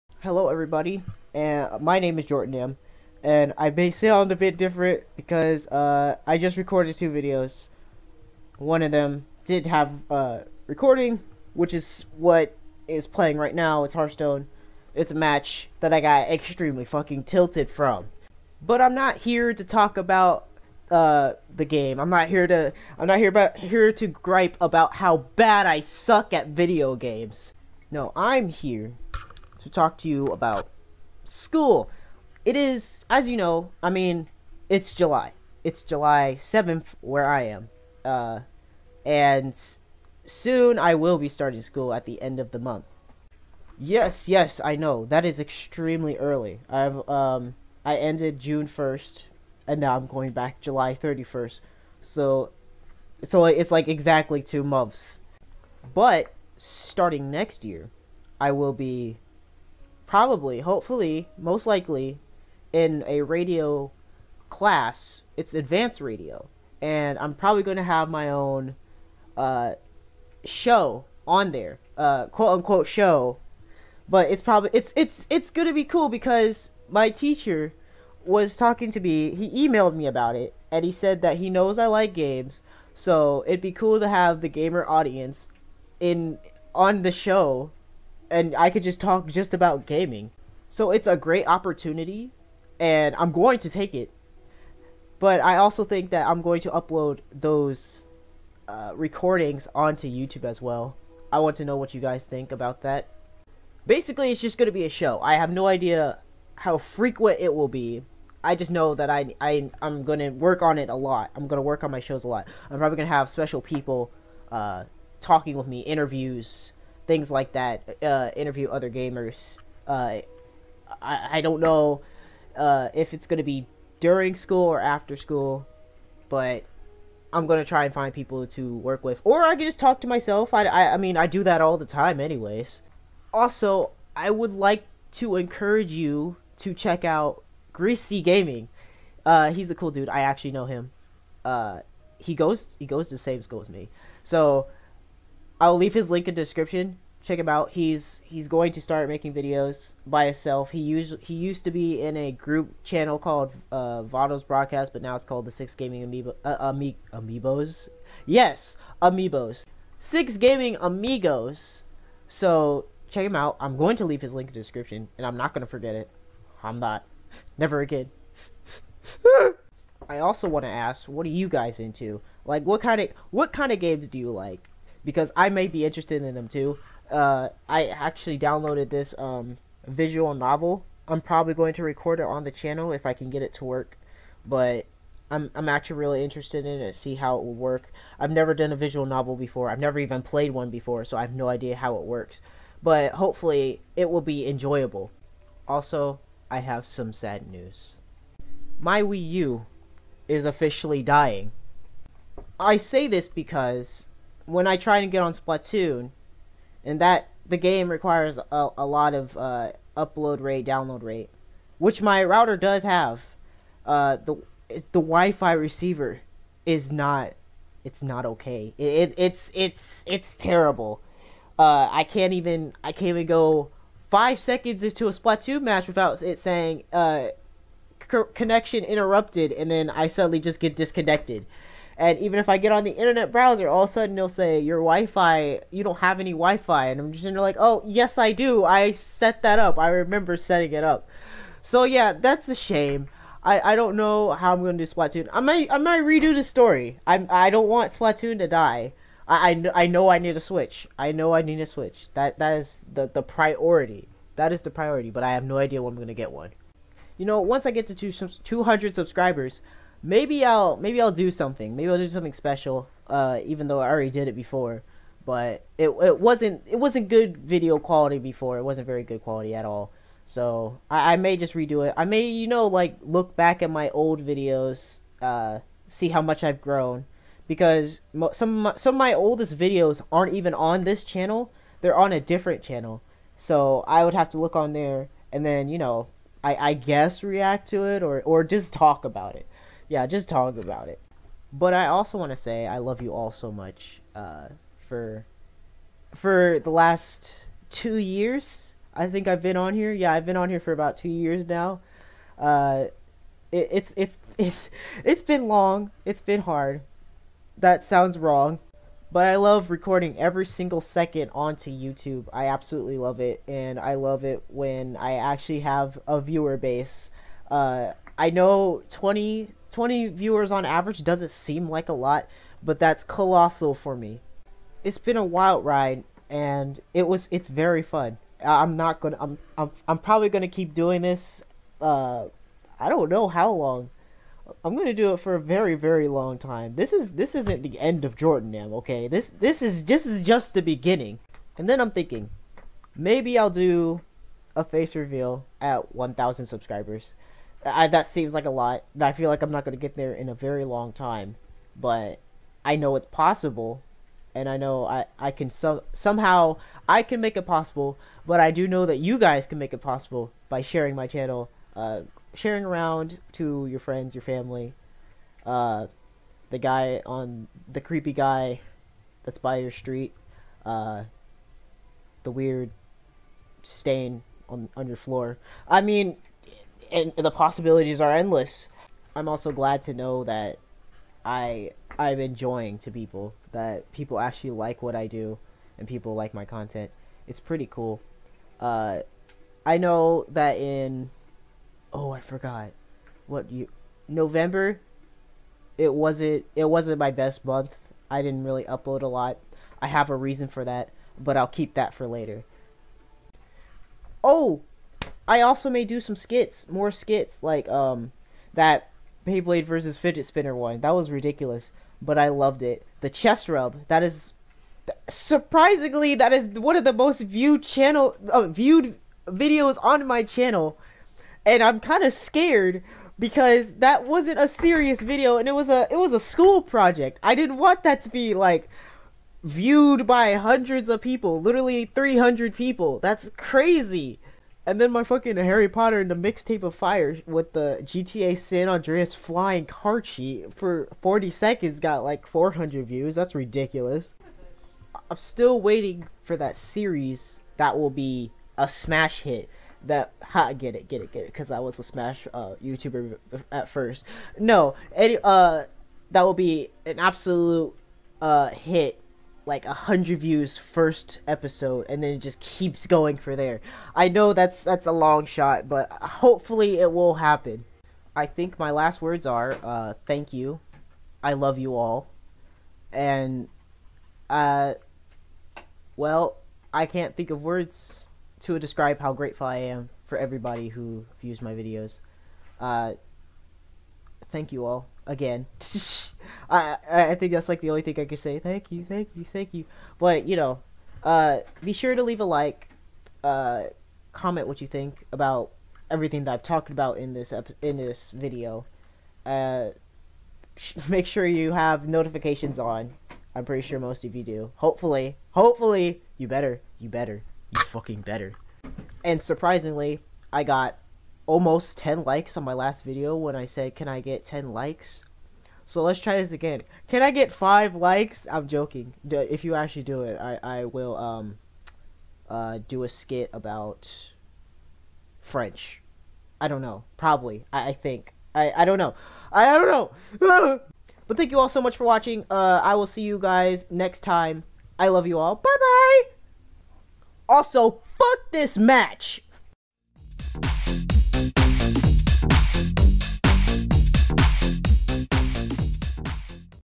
I'M GETTING A RADIO SHOW? (Commentary Video)
By fall I should have a new show set up. I'll keep you all in touch!

Nintendo Network ID: JordonAM
Nintendo Network Friend Code; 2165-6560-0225